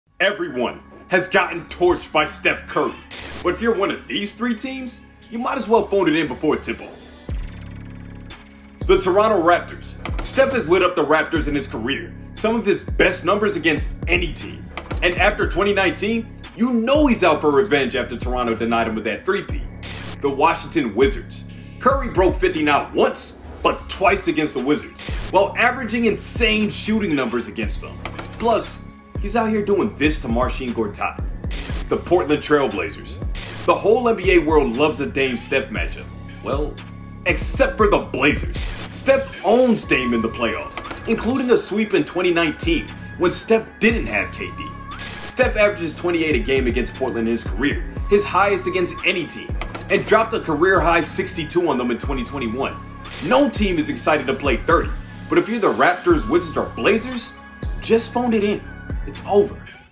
3 Teams That HATE Playing Steph Curry | Clutch
No one looks forward to playing a Golden State Warriors team that has Stephen Curry, Klay Thompson, and Draymond Green.
But the Toronto Raptors, Washington Wizards, and Portland Trail Blazers should be absolutely TERRIFIED.